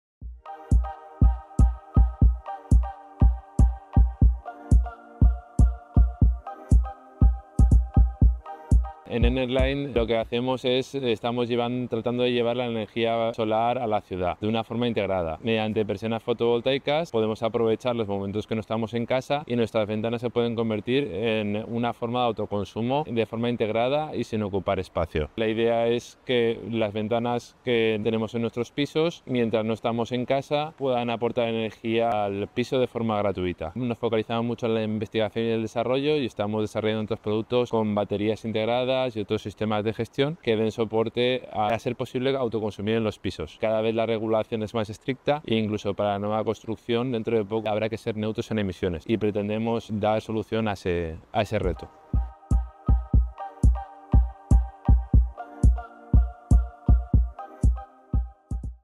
Premios VLC/Startup 2023 | ENERGY BLIND
Guillermo López nos presenta Energy Blind